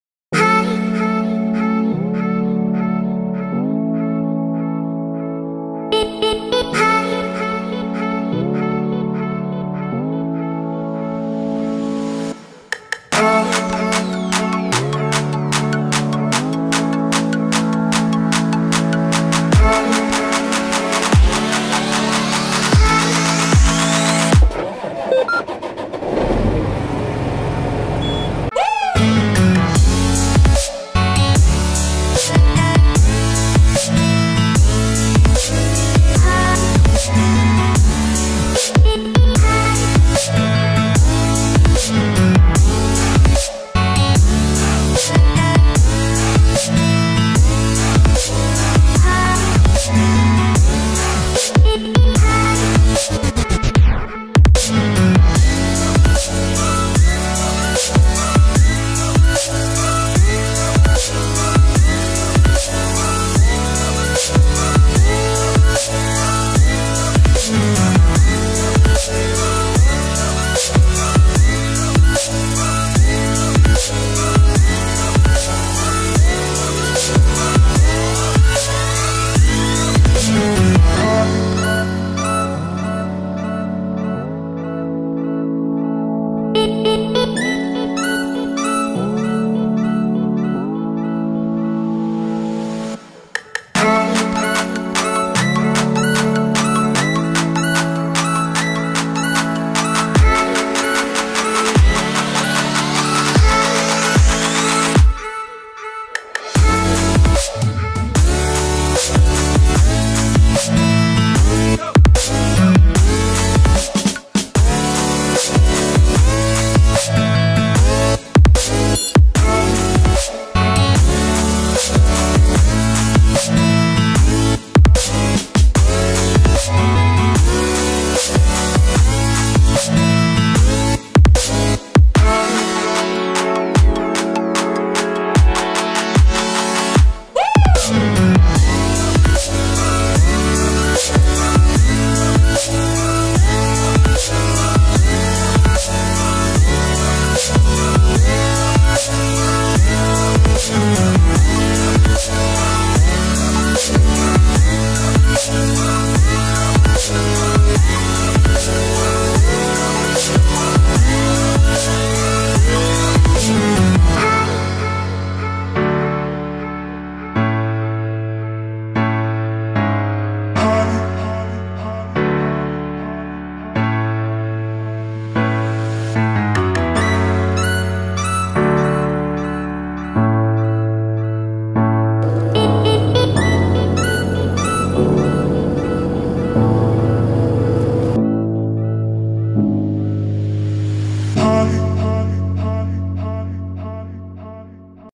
John Deere 8370R no plantio do algodão
Plantio do algodão na Fazenda Reata em Campo Novo do Parecis - MT

Conjunto John Deere 8370R com plantadora DB 31 linhas de 80cm

🔰Somos o maior portal de Máquinas Agrícolas do Brasil